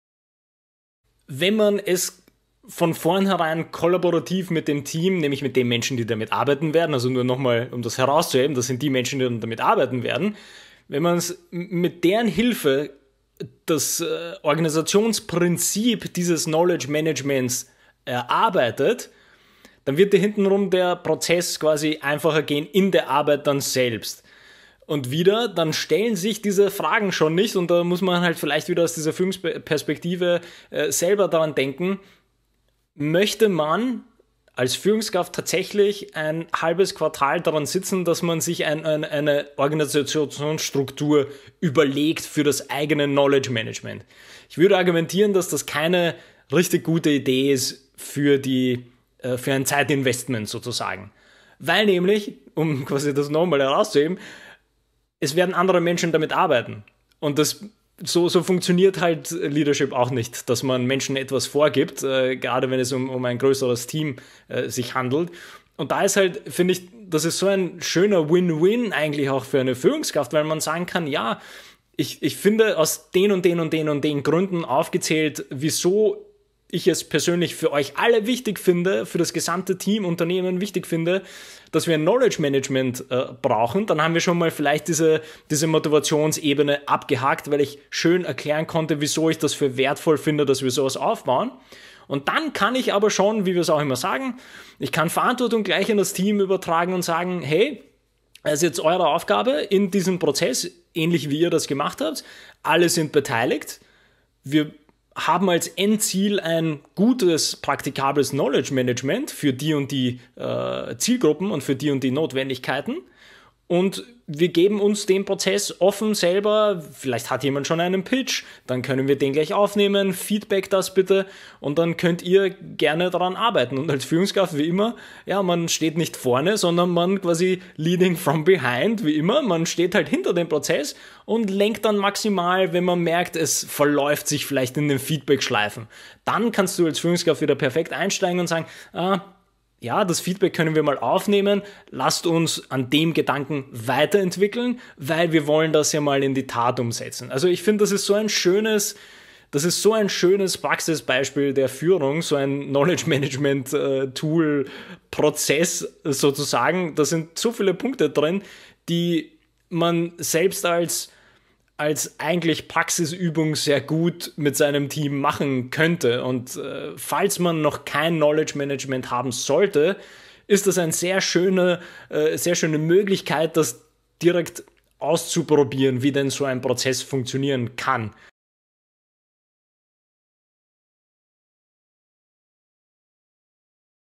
Knowledge Management als Lernmöglichkeit für Führungskraft
In diesem kurzen Clip spricht Bence über die Lernmöglichkeit für junge Führungskraft, indem der Aufbau eines Knowledge Management Systems betreut wird.